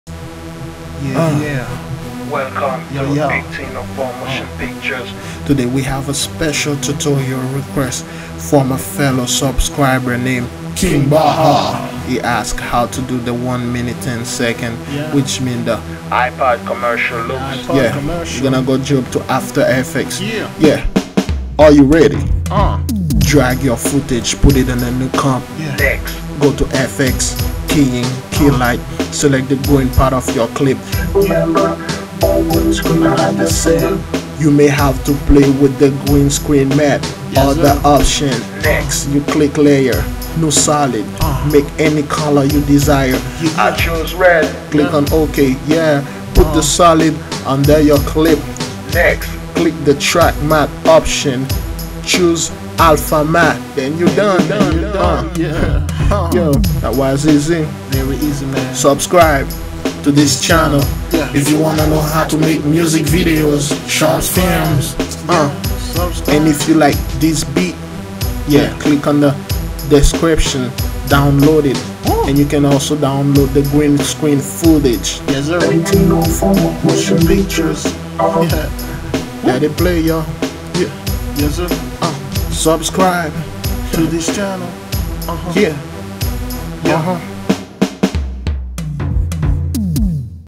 How to make the Ipod commercial looks in After Effects CC
How to make the Ipod Commercial looks in After effects
the key to this effects is green screen first you need follow this steps
1-Drag your footage into the new comp button
2-Go to effects choose Keylight then Keylight 1.2
3-Drop it on your video
4-Create a new solid, make it whatever color you desire
5- Click OK
6-Put the solid under the footage
7-on the solid clip go to track matte and select Alpha on the drop list.
8-Then you done
Music video effects